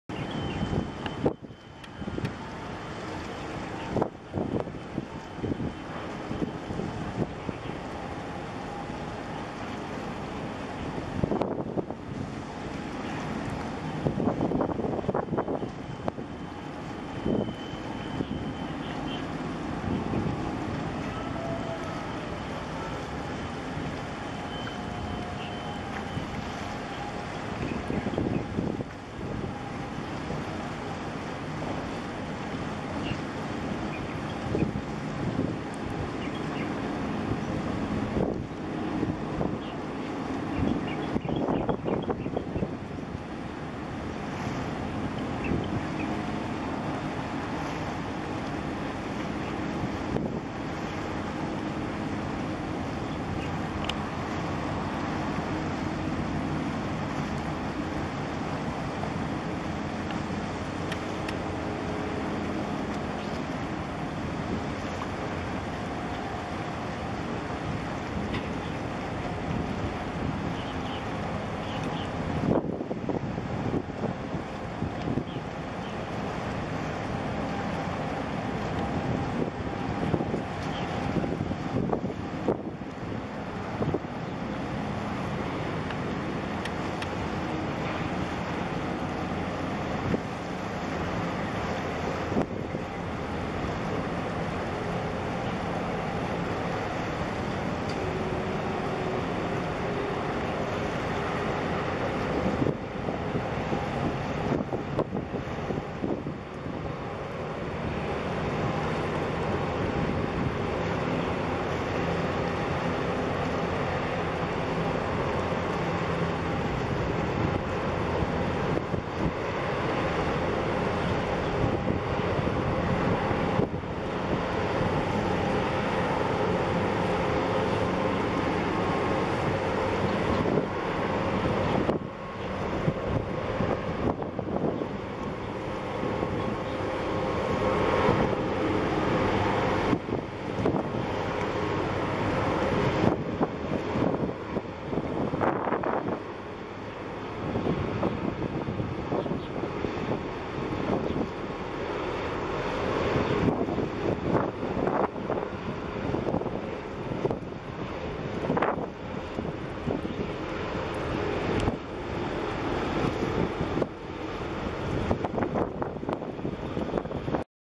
11/2 中遠東方英格蘭 COSCO ENGLAND 第7趟來高雄 (碼頭1066→S2出港) 船籍香港 航線South China Pacific Service(PVCS)華南太平洋服務
前一港:香港 次一港:美國長堤

IMO 9516428
Callsign VRML8
Flag (Registration)Hong Kong
Gross Tonnage153666
Deadweight (t)156618
Length (m)366
Beam (m)51.2
TEU 13386
Built (year) AUG-30-2013
Builder COSCO NANTONG SHIPYARD
Yard NANTONG
Owner COSCO ENGLAND SHIPPING LIMITED